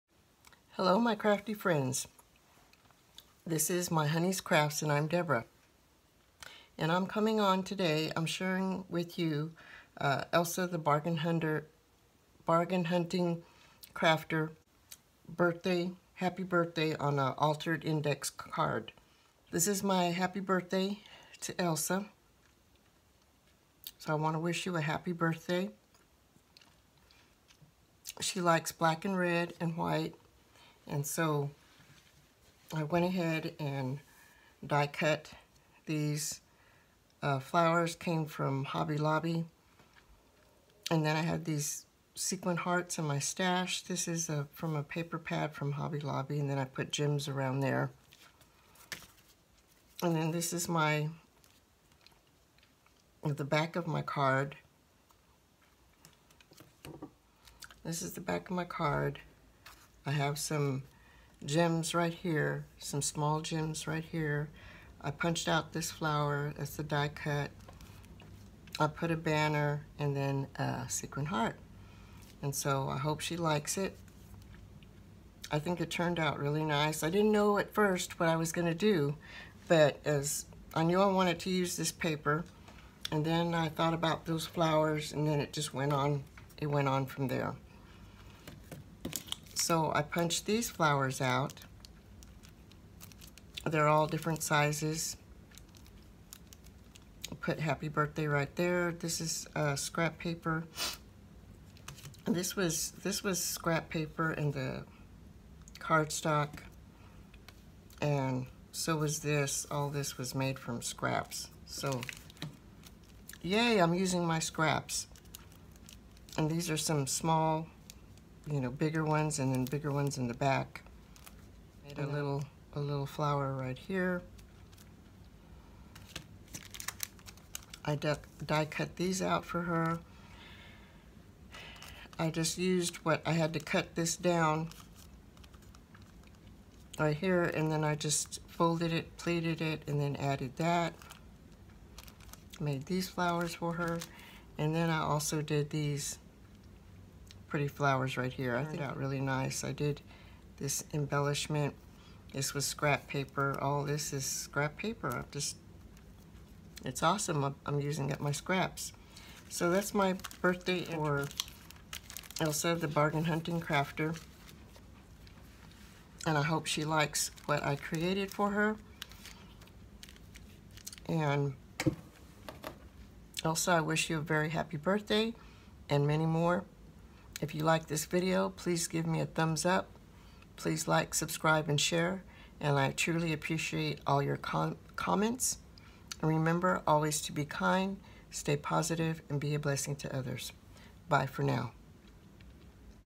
What I created for Elsa The Bargain Hunting Crafter
Welcome to My Honey’s Crafts. Thanks for stopping by…I created an altered index card for Elsa The Bargain Hunting Crafter. She requested the altered index card for her 50th birthday. Please stop by her channel and show her some crafty love and wish her Happy Birthday. Tell her I sent you.

Take care,
Debra
My Honey’s Crafts🌺🌹🌻🌼